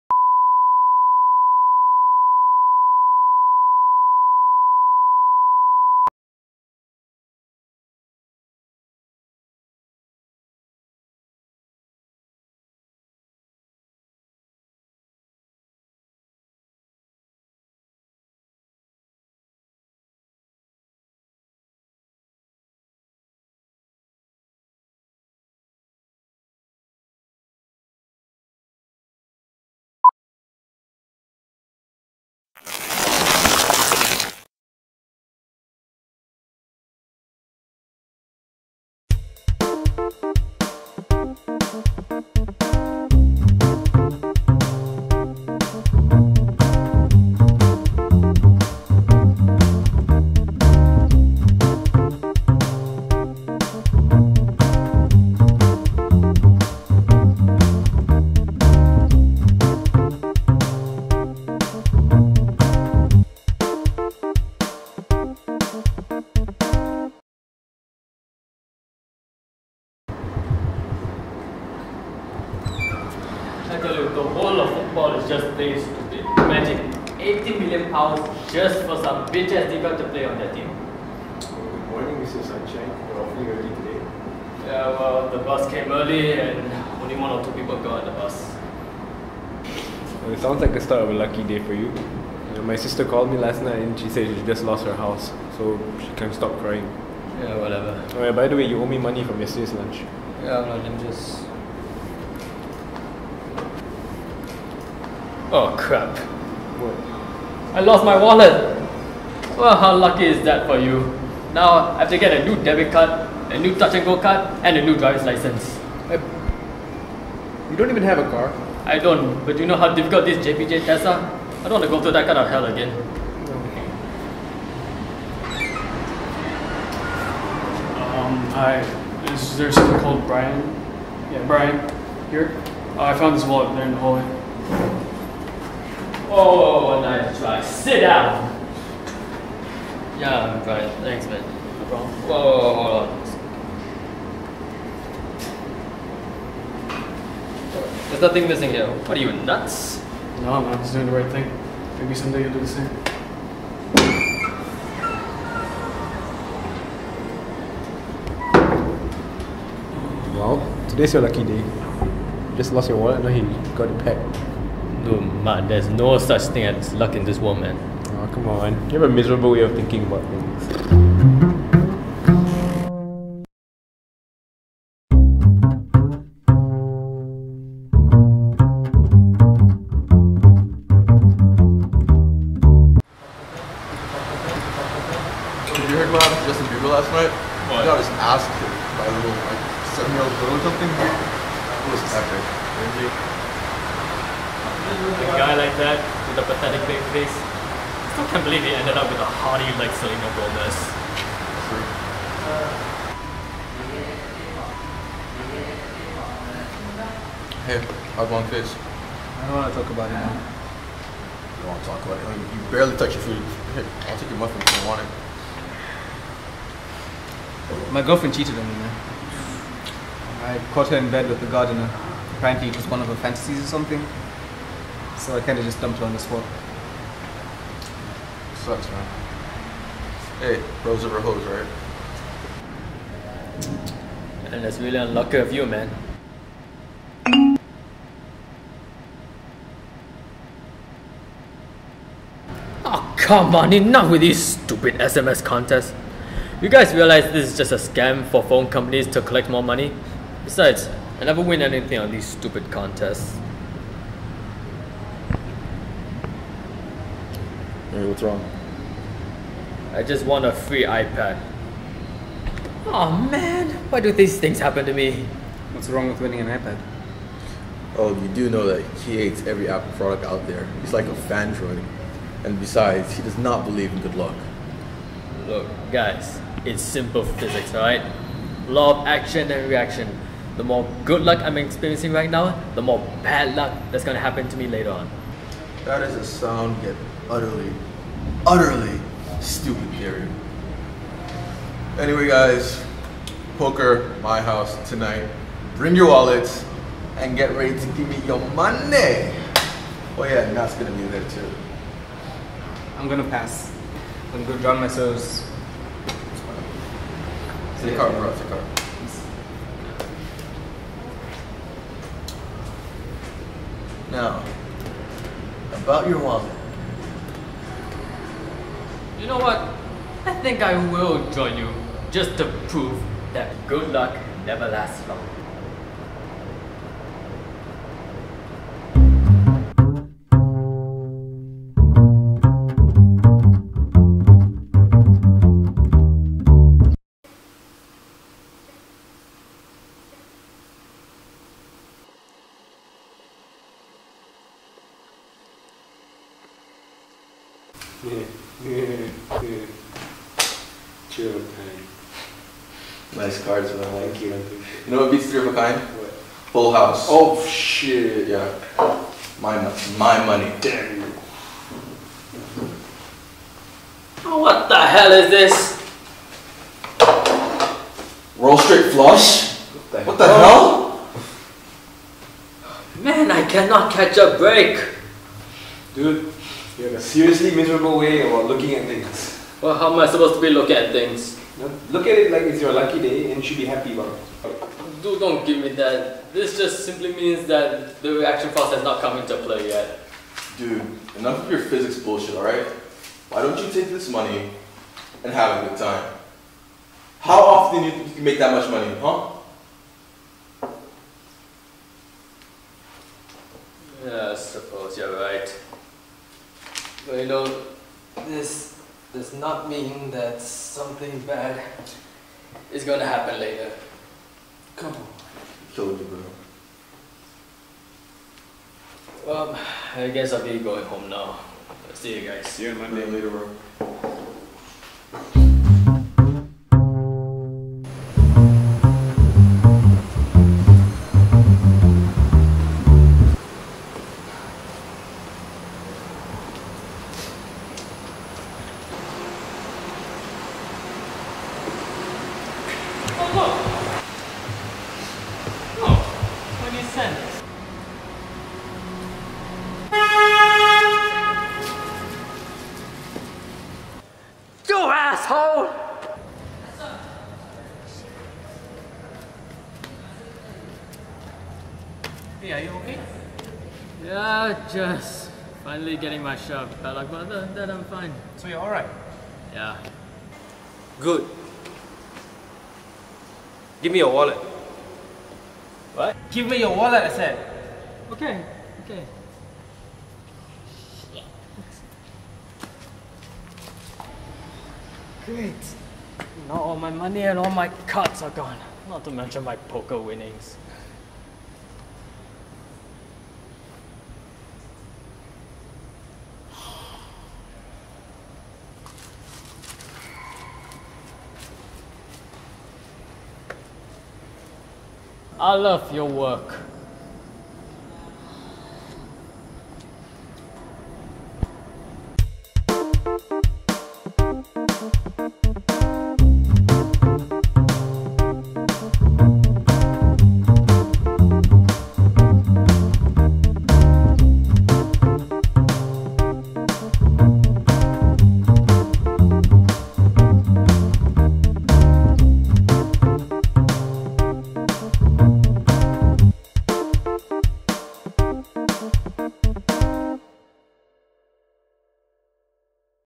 Lucky Friday - 20 November, 2012.mp4
Another short film for this semester.  A simple story about a pessimistic guy who experiences a day of good luck, only to lose it all at the end of the day.

Still can't believe my script was 1 of the 5 chosen out of an entire class of 30 students.  The roles I have taken in this project are the main actor, writer, and editor.  The music is composed by me along with another guy.  Also partially directed by me but credit is given entirely to another person.